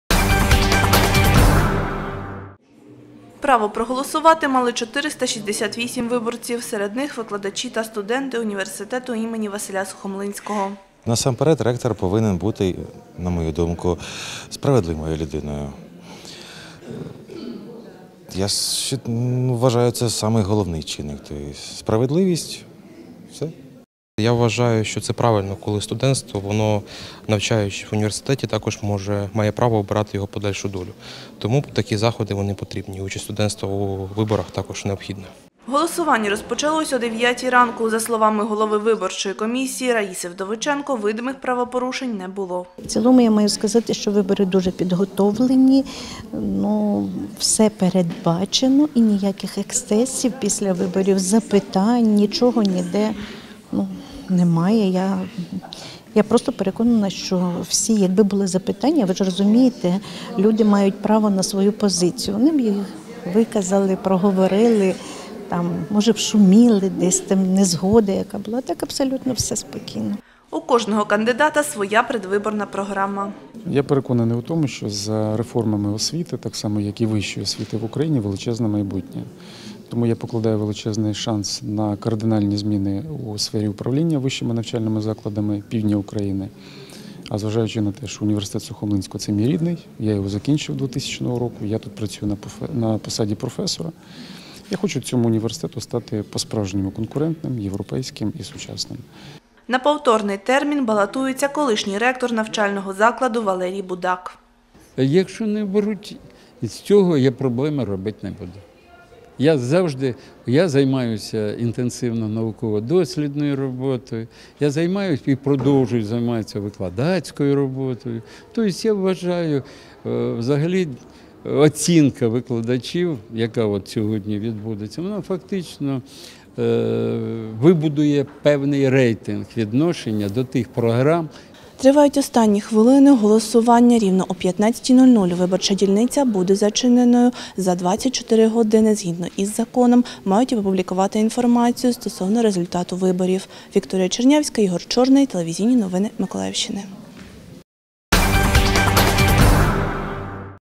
Вибори ректора в університеті імені Василя Сухомлинського
У Миколаївському національному університеті імені Василя Сухомлинського відбулись вибори ректора. За посаду керівника змагалось семеро кандидатів.
Автор: В. Чернявська.